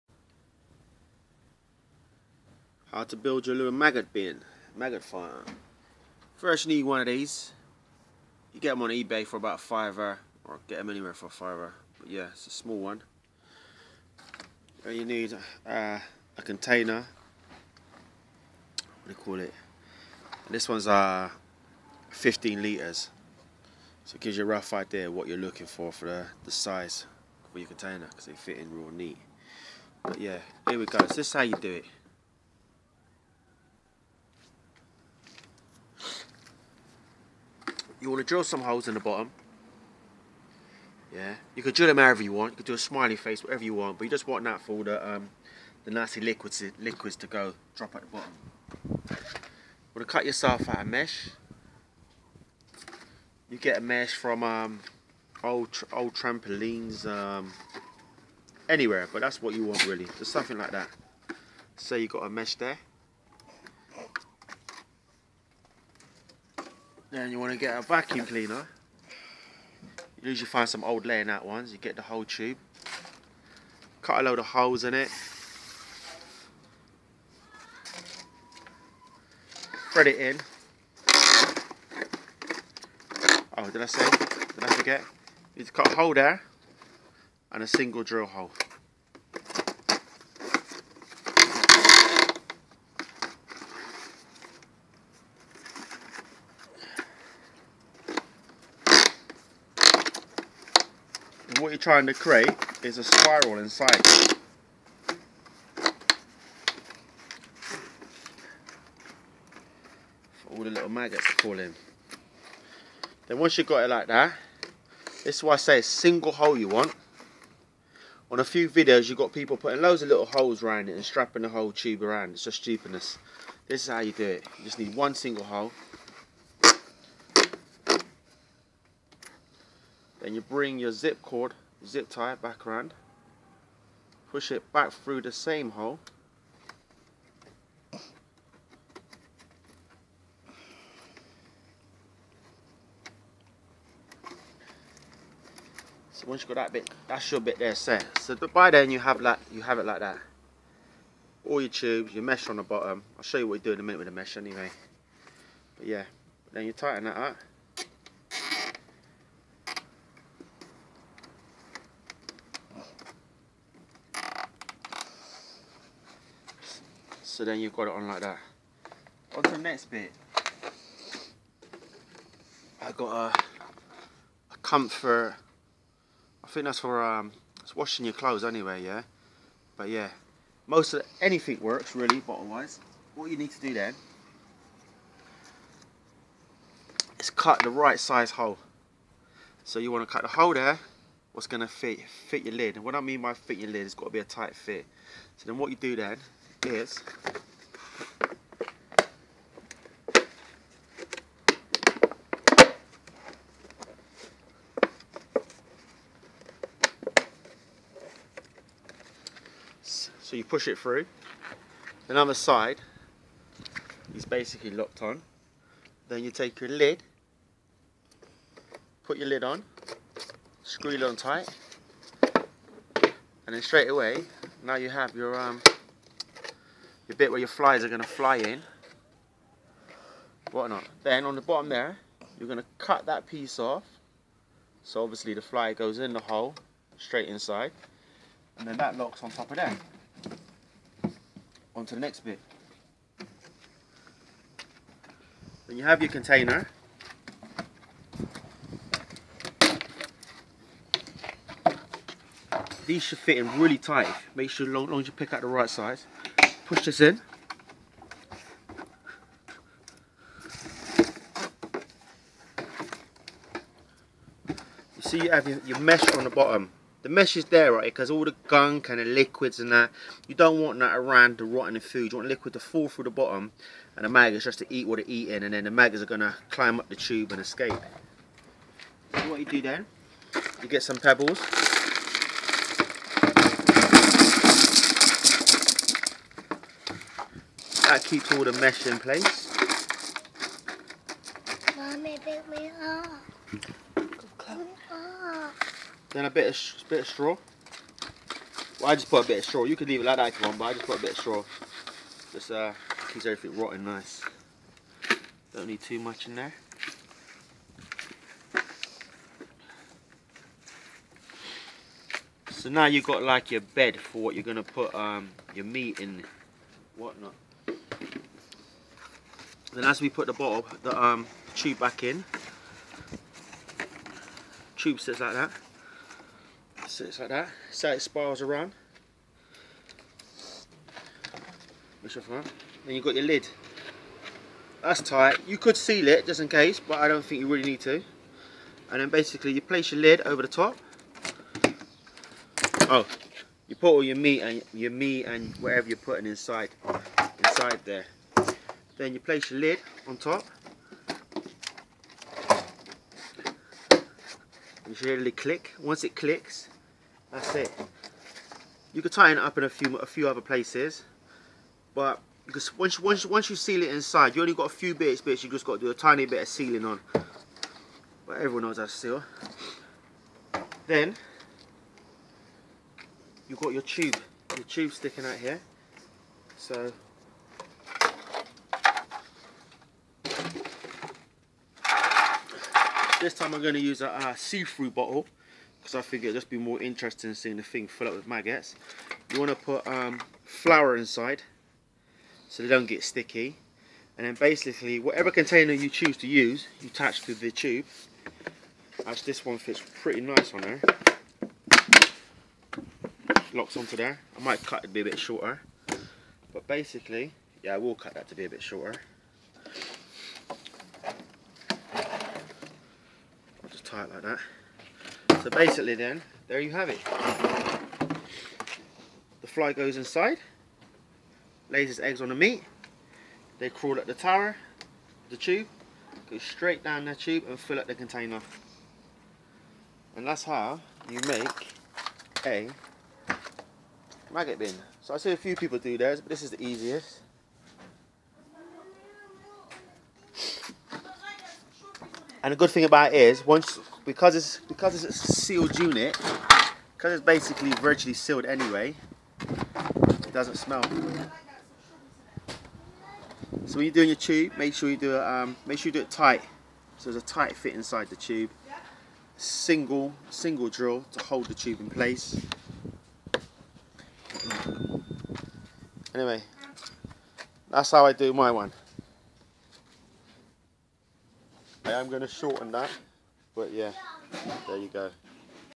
How to Build a Maggot Farm The Right Way: Feed Fish & Poultry
Best way for cheap effective maggot beeding for backyard maggot farm great for poultry fishing ect should take you half an hour give or take